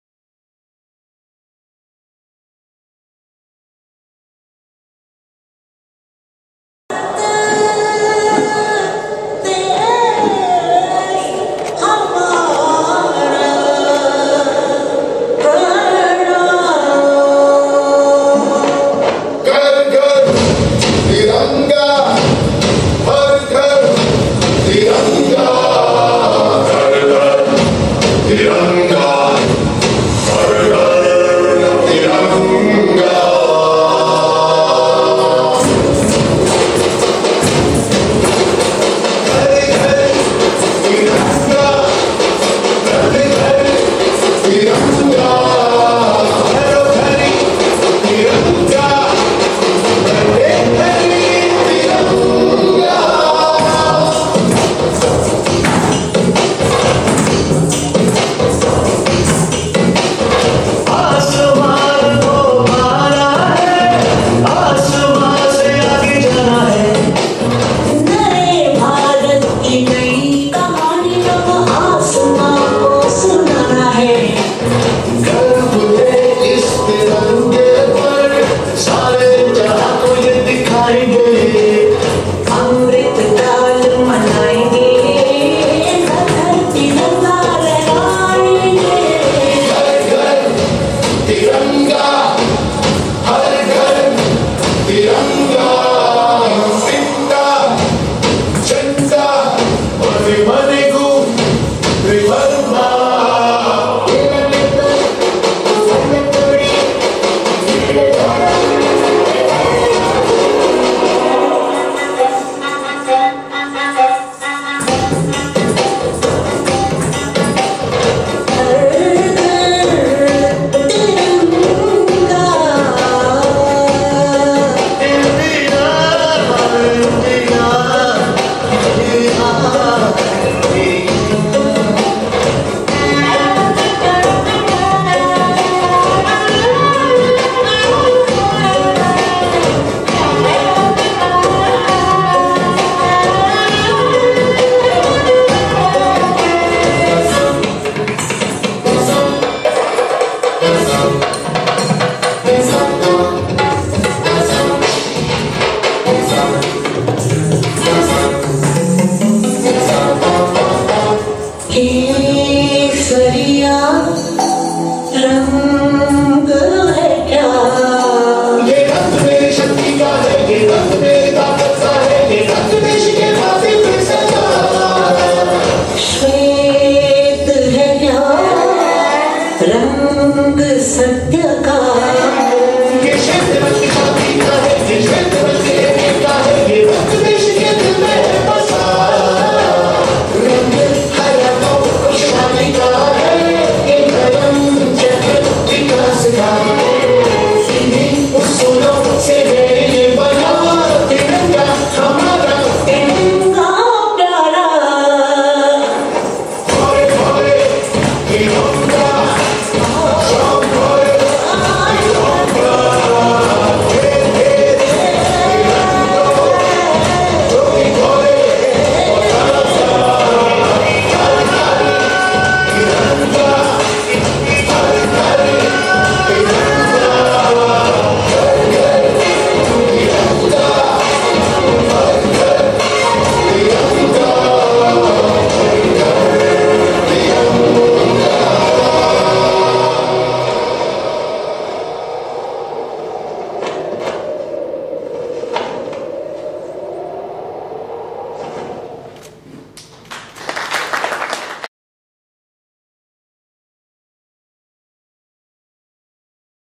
Solo theatre festival
Kerala Sangeeta Nataka Academy which is working under the department of Culture, Government of Kerala  in association with Darsana Kalakendra has conducted a Solo theatre festival  at Government Tribal High School Auditorium, Idukki to mark Har Ghar Tiranga.